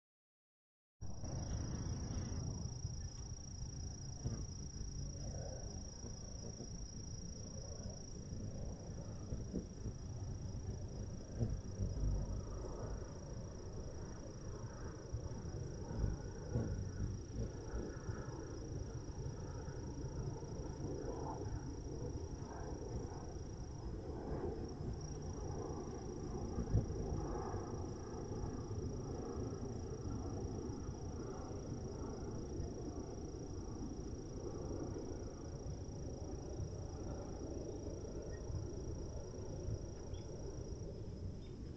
Hummingbird At Beehive
We frequently see the bees around the hummingbird feeder, but this is the first time we've seen the hummingbird visit a hive. It doesn't seem to be interested in the bees, only a substance on the front of the hive.